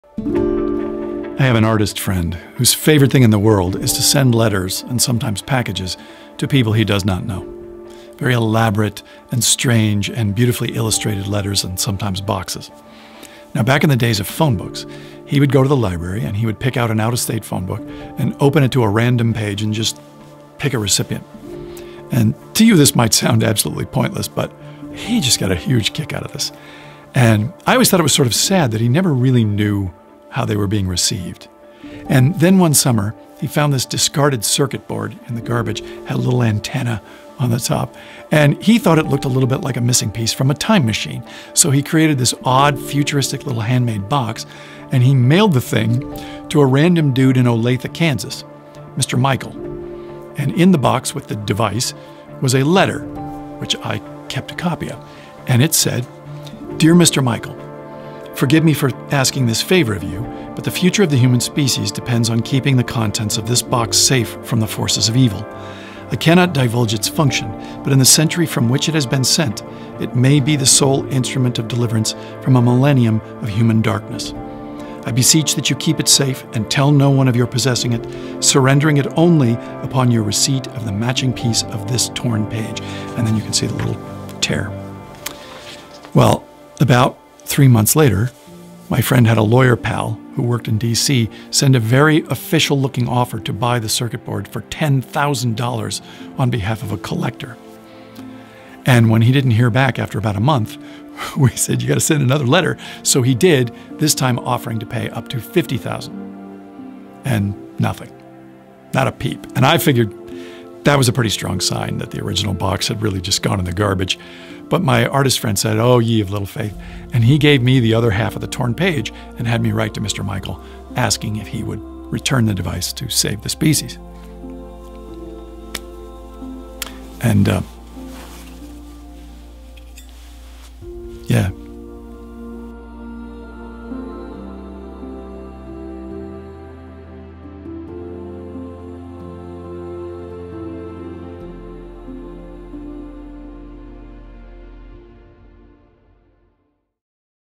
Random Saviors
A story celebrating the quiet people willing to sacrifice to protect people they know they will never meet.